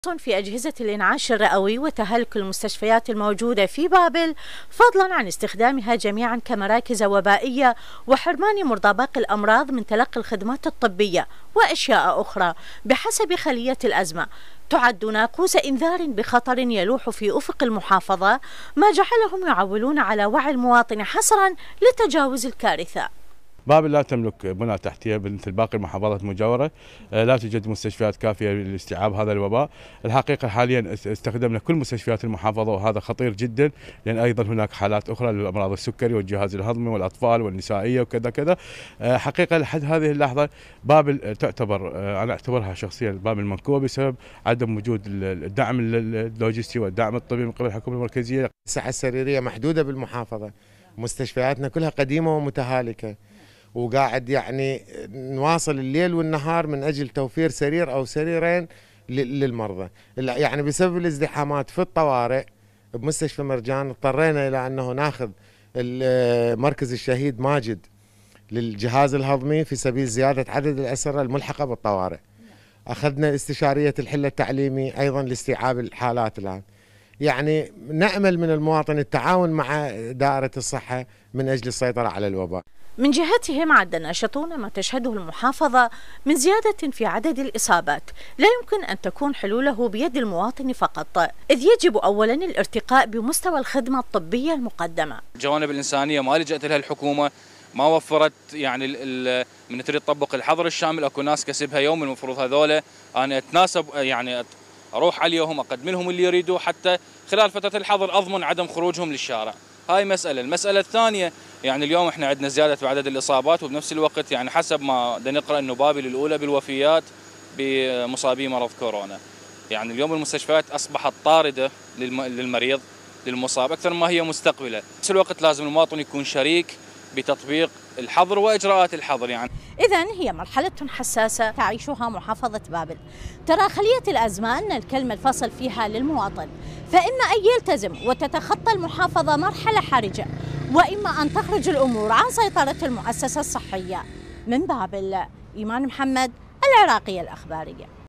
بابل - المحافظة تدخل مرحلة حرجة من تفشي جائحة كورونا - ايمان محمد
نشرة السادسة مع علي الربيعي